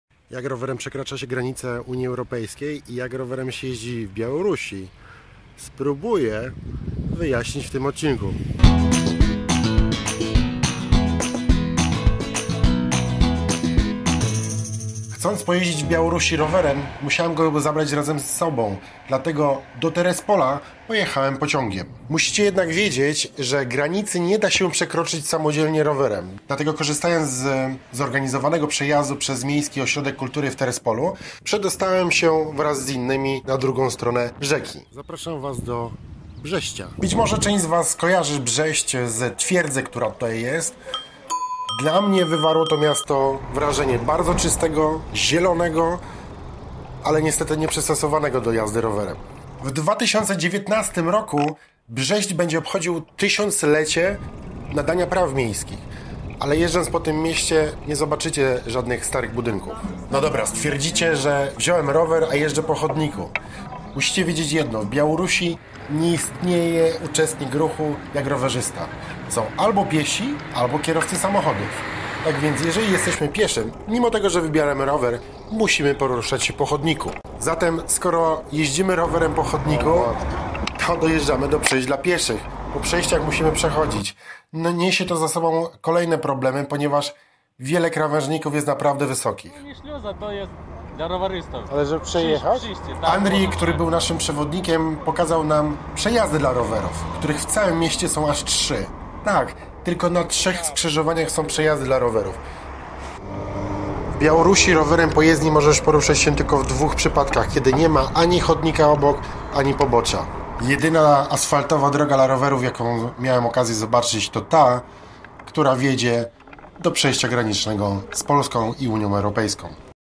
Jak się jeździ rowerem w Białorusi? - 2 minuty o 2 kołach
Czy można przekroczyć granicę rowerem? Jak się jeździ rowerem po wschodniej stronie granicy? Czy jest po czym jeździć?